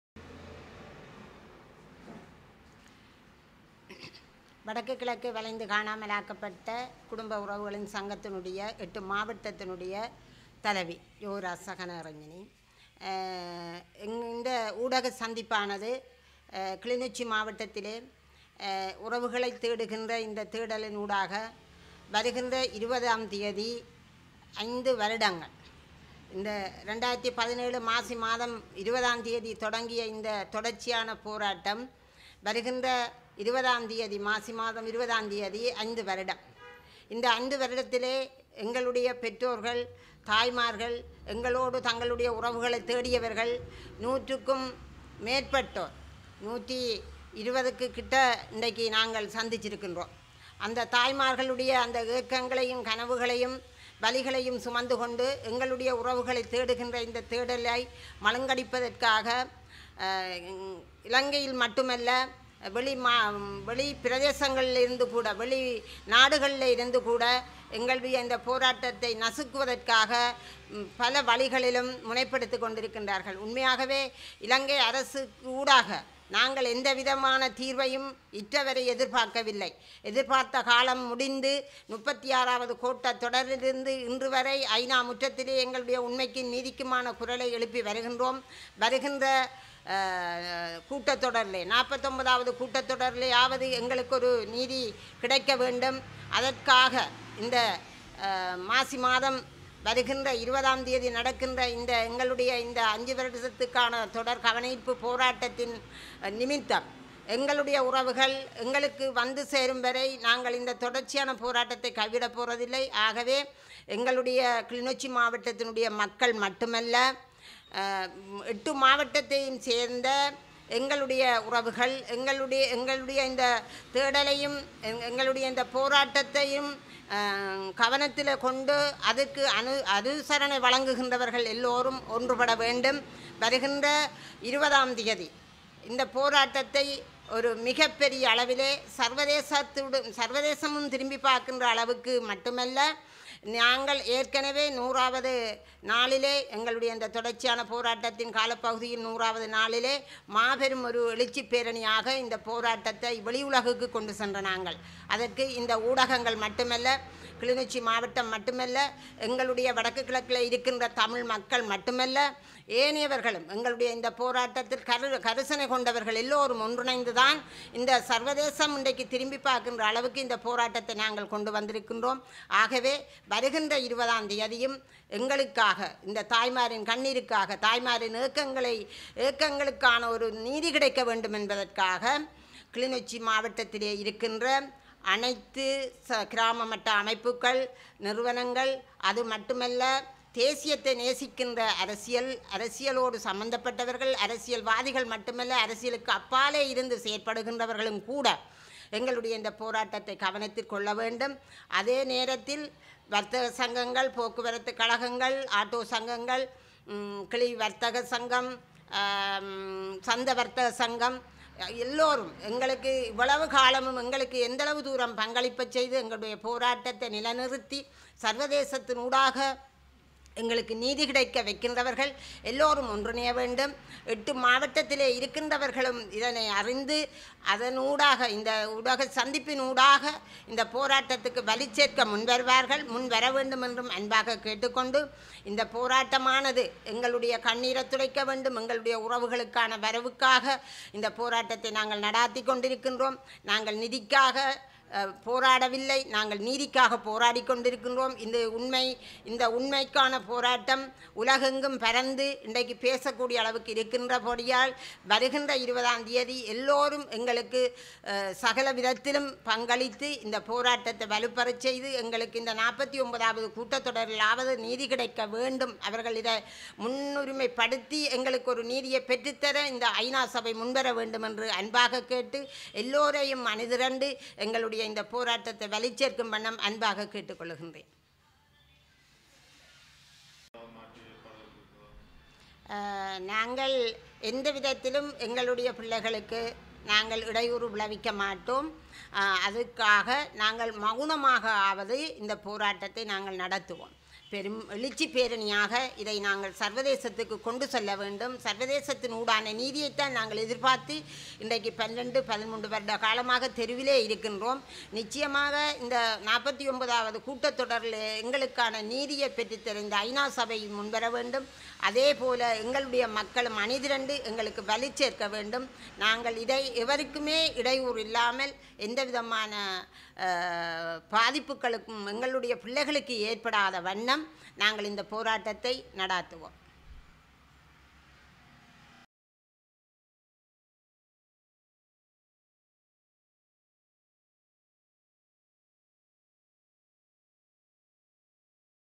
மாபெரும் போராட்டத்திற்கு வலிந்து காணாமல் ஆக்கப்பட்டவர்களின் சங்கம் அழைப்பு
எதிர்வரும் 20ம் திகதி மாபெரும் போராட்டத்திற்கு வலிந்து காணாமல் ஆக்கப்பட்டவர்களின் சங்கம் அழைப்பு விடுத்துள்ளது. கிளிநொச்சி வலிந்து காணாமல் ஆக்கப்பட்டவர்களின் உறவுகளின் சங்க தலைவி யோகராசா கனகரஞ்சினி குறித்த அழைப்பினை இன்று ஊடகங்கள் வாயிலாக அறிவித்துள்ளார்.